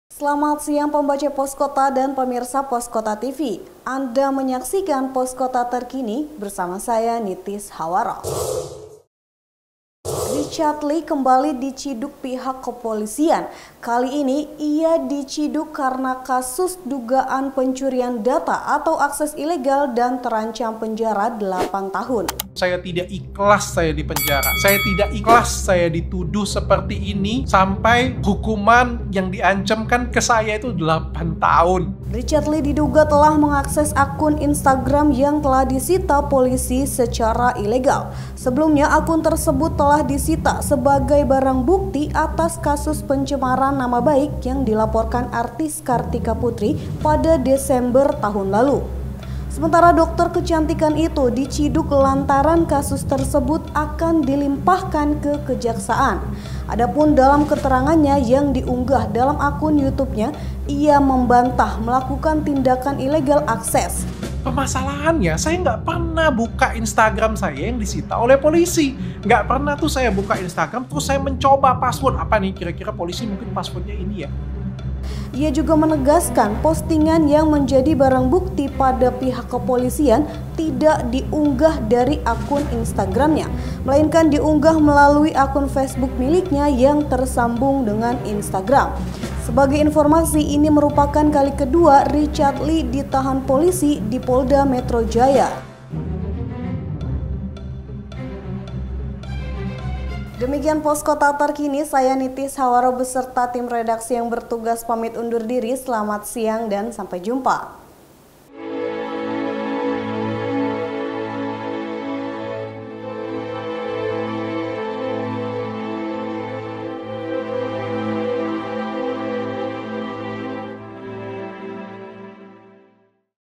POSKOTA TERKINI: RICHARD LEE KEMBALI DICIDUK POLISI ATAS DUGAAN PENCURIAN DATA ILEGAL
POSKOTA TERKINI EDISI SELASA SIANG 28/12/21

Poskota Terkini menyajikan beberapa berita menarik yang wajib anda simak.

Richard Lee kembali diciduk pihak kepolisian, kali ini ia ditangkap karena kasus dugaan pencurian data atau akses ilegal dan terancam penjara 8 tahun.

Video: Youtube dr. Richard Lee, MARS